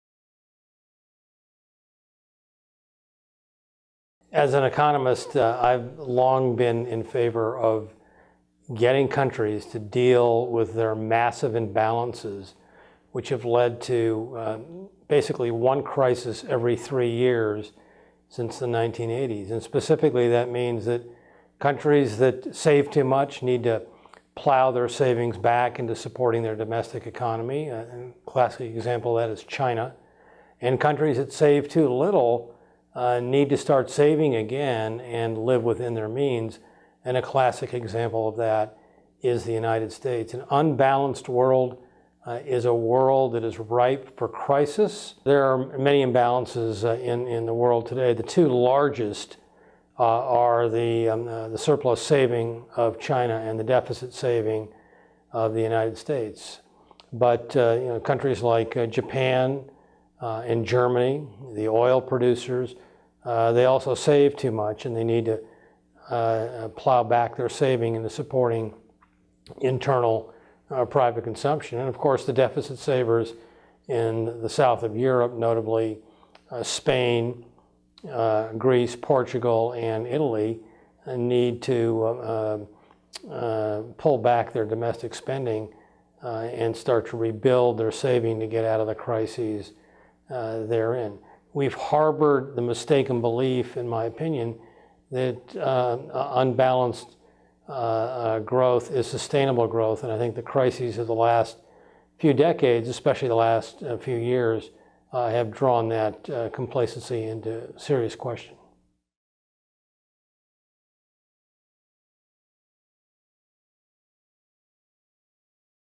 Stephen Roach: Averting Another Economic Crisis
Stephen Roach, Yale Jackson Institute Senior Fellow and Senior Lecturer at Yale's School of Management, explains the unsustainability of an unbalanced world economy, with specific references to the US and China. This video is part of the Yale Global Perspectives series.